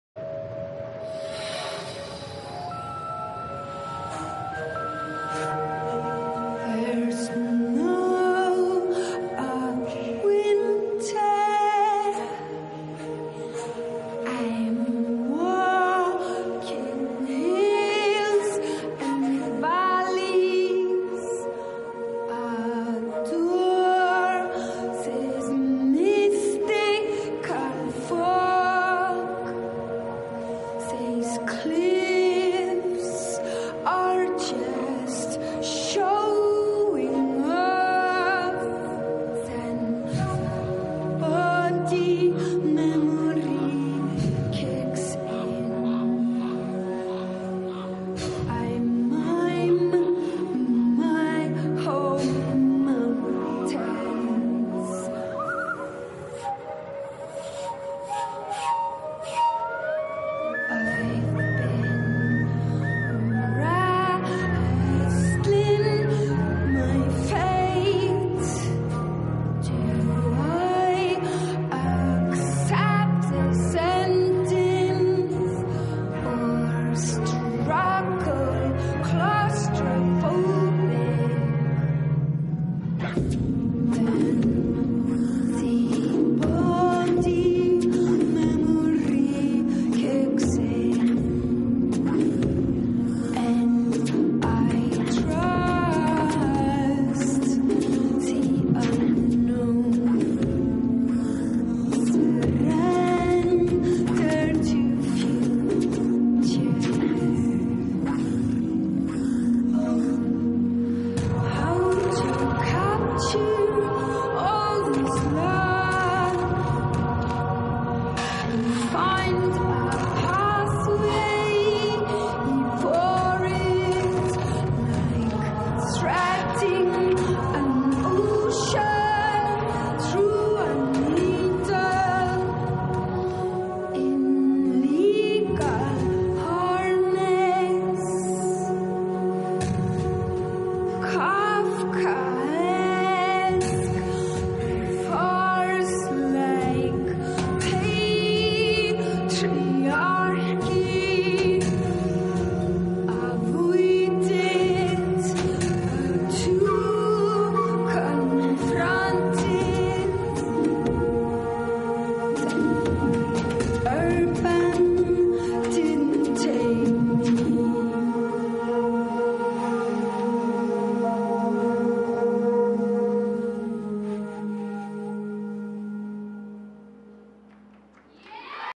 Elliana Mannella- Body Memory
1st Overall + Dj's Pick
Nuvo Minneapolis 2018
Choreo: Talia Favia